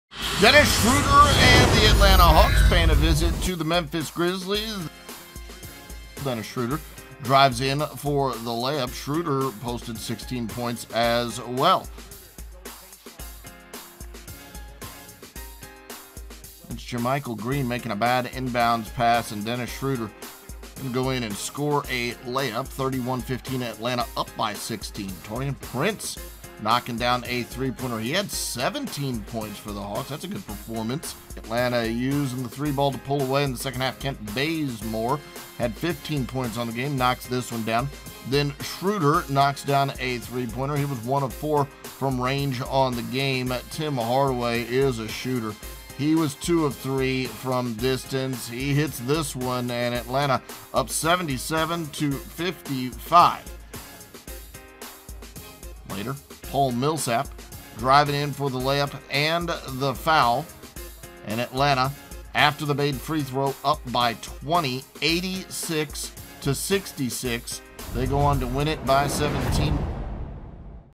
Atlanta Hawks at Memphis Grizzlies - March 11, 2017
Paul Millsap get a double-double as the Hawks defeat the Grizzlies, 107-90.